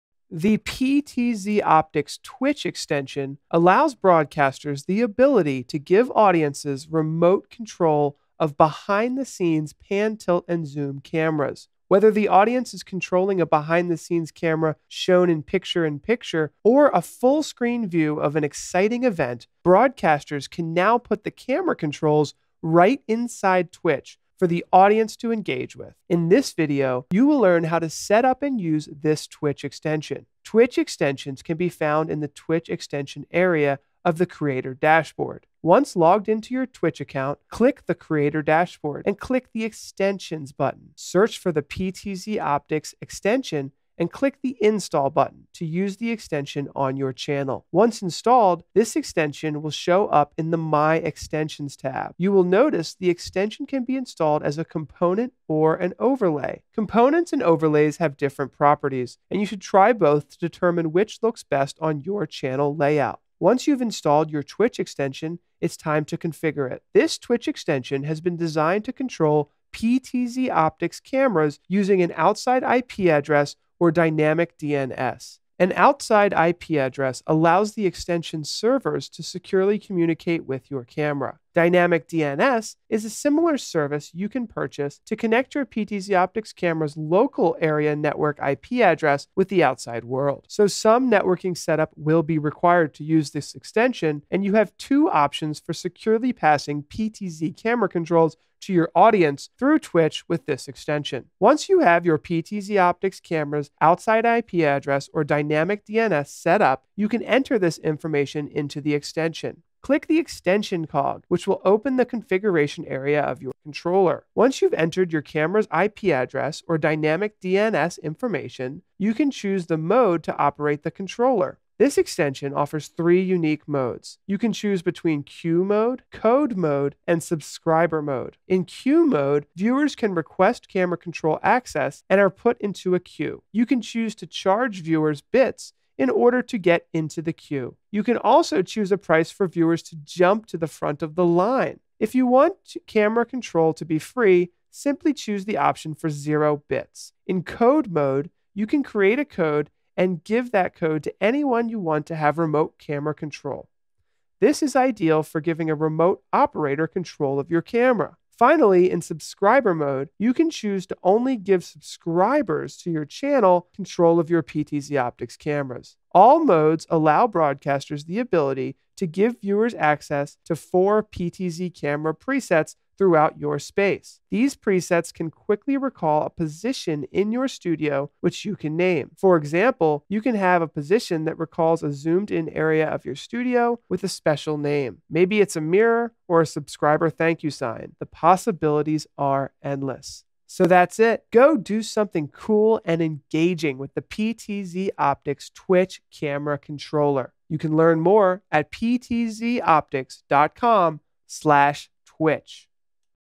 PTZOptics Twitch Camera Extension - Bits Enabled!
The new PTZOptics Twitch extension allows broadcasters the ability to give audiences remote control of a behind the scenes pan, tilt, and zoom camera. Whether the audience is controlling a behind the scenes camera shown with picture-in-picture or a full-screen view of an exciting event, broadcasters can put the camera controls right inside Twitch for the audience to engage with. In this video, you will learn how to set up and use this Twitch extension. Twitch extensions can be found in the Twitch extension area. Once logged into your Twitch account. Click the “Creator Dashboard” and click “Extensions”. Search for PTZOptics and click the “install” button to use the extension on your channel.

Once installed the extension will show up in the “My Extensions Tab.” You will notice the extension can be installed as a component or an overlay. Components and overlays have different properties and you should try both to determine which looks best on your channel.

Once you have installed your Twitch extension it’s time to configure it. This Twitch extension has been designed to control PTZOptics cameras using an outside IP address or dynamic DNS. An outside IP address allows the extension servers to communicate with your camera and can be purchased from your internet service provider. Dynamic DNS is a service you can purchase to connect your PTZOptics camera's local area network IP address with the outside world. So you have two options for securely passing PTZ controls to your audience through Twitch with this extension.

Once you have your PTZOptics cameras outside IP address or Dynamic DNS setup you can enter this information into the extension. Click the extensions “cog” which will open the configuration area of your controller. Once you have entered your cameras IP address or Dynamic DNS info you can choose the mode to operate the controller. The extension offers three unique modes. You can choose between Que mode, Code mode, and Subscriber mode. In Que mode, viewers who request camera control access are put into a que. You can choose to charge the viewer's bits in order to get into the que. You can also choose a price for viewers to jump to the front of the line. If you want camera control to be free, simply choose the option for zero bits.

In code mode, you can create a code and give that code to anyone you want to have control of your camera. This is ideal for having a remote camera operator. Finally, in subscriber mode, you can choose to only give subscribers on your channel control of y our PTZOptics camera.

All modes allow broadcasters the ability to give viewers access to 4 PTZ camera presets. These presets can quickly recall a position in your studio which you can name. For example, you can have a position that recalls a zoomed-in area of your studio with a special name. Maybe it is a mirror, or a subscriber thank you sign. The possibilities are endless.